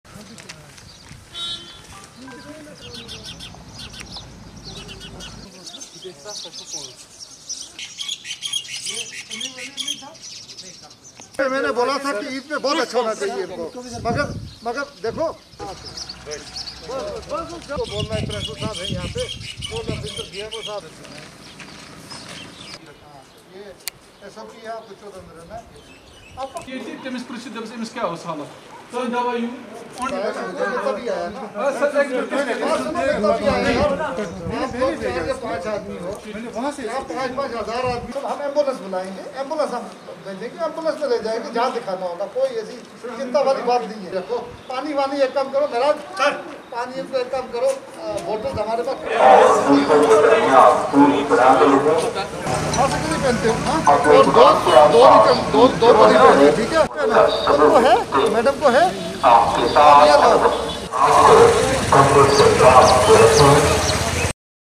DC Ganderbal reviews implementation of guidelines of lockdown 4.0,
DC Ganderbal reviews implementation of guidelines of lockdown 4.0, inspects
availability of facilities at administrative quarantine centre